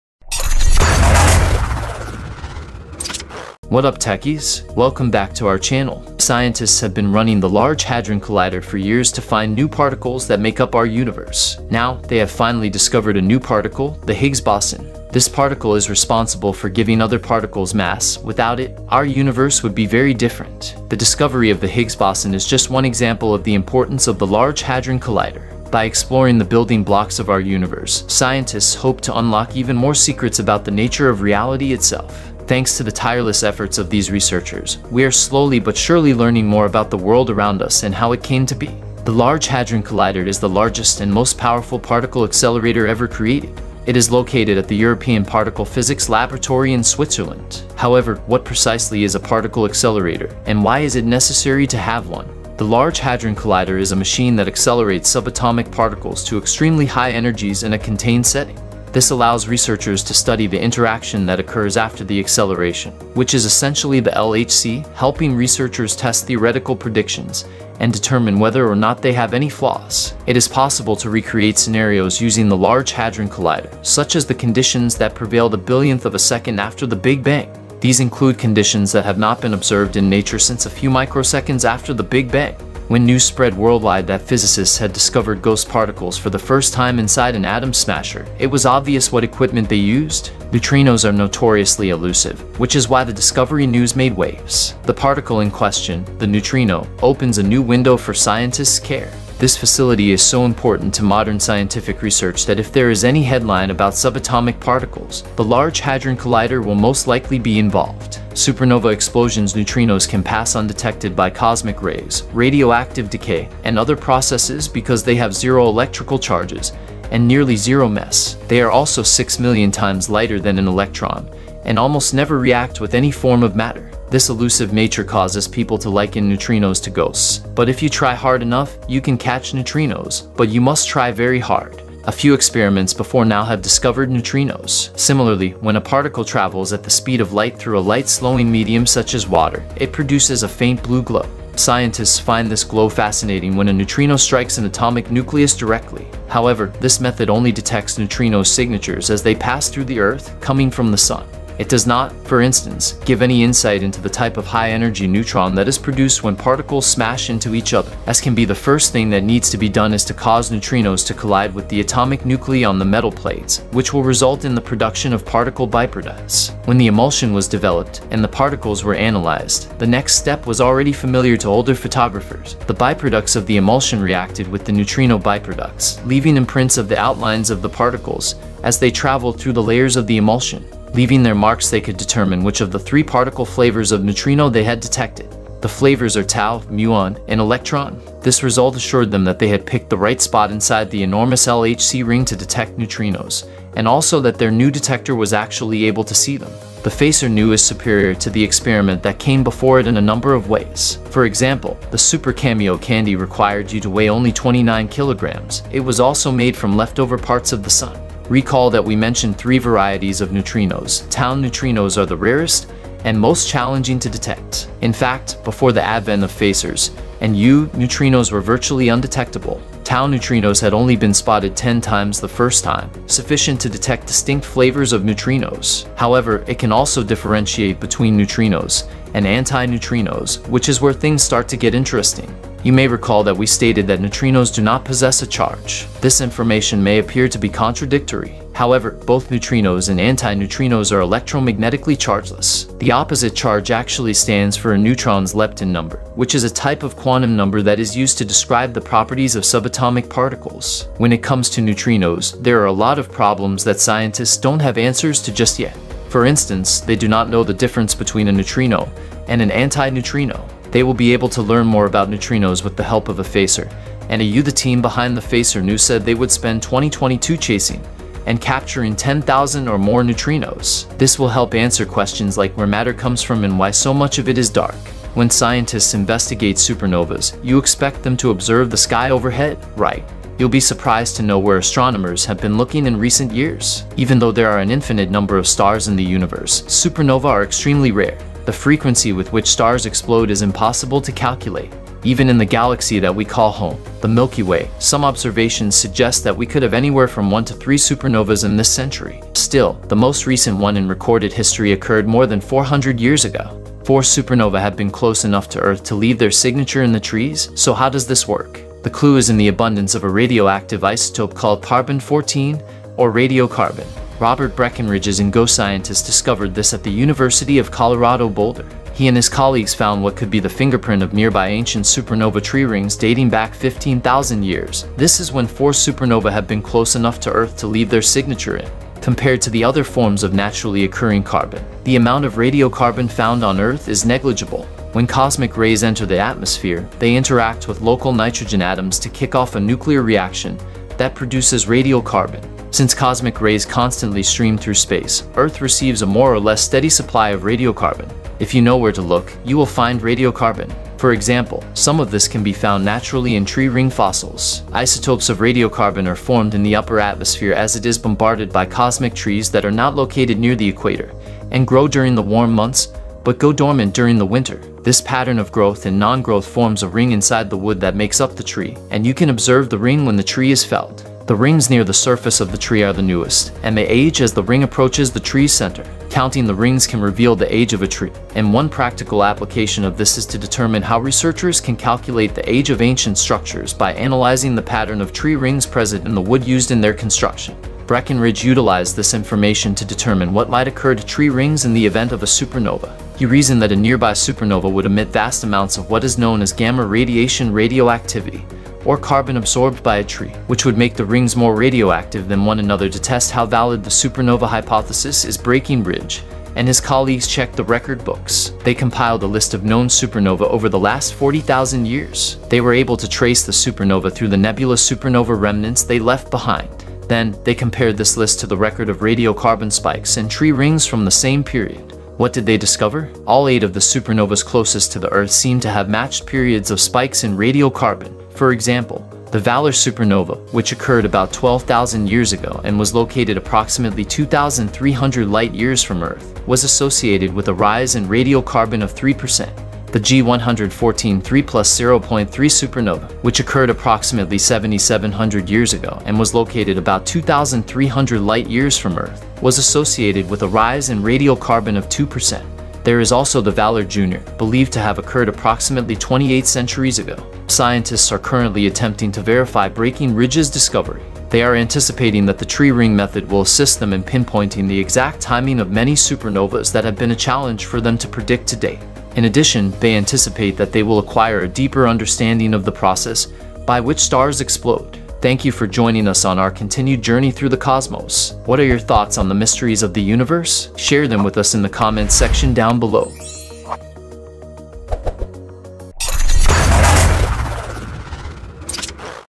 Did The Large Hadron Collider Just Find Something incredible?!
What up Techies! Welcome Back! Did The Large Hadron Collider Just Find Something incredible?! Why Are Scientists Excited About The Large Hadron Collider Finding Something For the First Time Ever?! Scientists have been running the Large Hadron Collider for years to find new particles that make up our universe. Now, they have finally discovered a new particle: the Higgs boson. This particle is responsible for giving other particles mass; without it, our universe would be very different. The discovery of the Higgs boson is just one example of the importance of the Large Hadron Collider. By exploring the building blocks of our universe, scientists hope to unlock even more secrets about the nature of reality itself. Thanks to the tireless efforts of these researchers, we are slowly but surely learning more about the world around us and how it came to be. In addition, they anticipate that they will acquire a deeper understanding of the process by which stars explode. What are your thoughts on the mysteries of the universe? Share them with us in the comment section down below. physicists